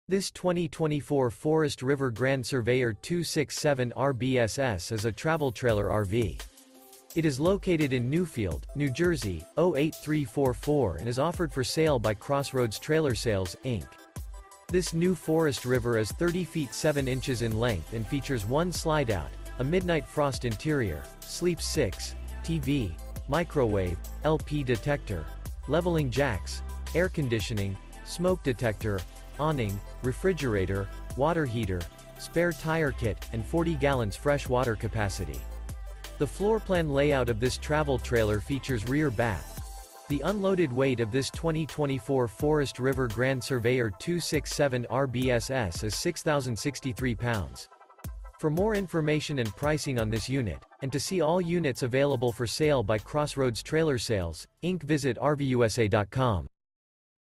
Amazing 2024 Forest River Grand Surveyor Travel Trailer RV For Sale in Newfield, NJ | RVUSA.com
This 2024 Forest River Grand Surveyor 267RBSS is a Travel Trailer RV. It is located in Newfield, NJ 08344 and is offered for sale by Crossroads Trailer Sales, Inc. This New Forest River Is 30' 7" in length and features 1 slideout, a Midnight Frost interior, sleeps 6, Stove Top Burner, Refrigerator, Smoke Detector, CO Detector, Awning, LP Detector, Oven, Water Heater, External Shower, Auxiliary Battery, and 40 gal fresh water capacity. The floorplan layout of this Travel Trailer features Rear Bath.The unloaded weight of this 2024 Forest River Grand Surveyor 267RBSS is 6063 lbs.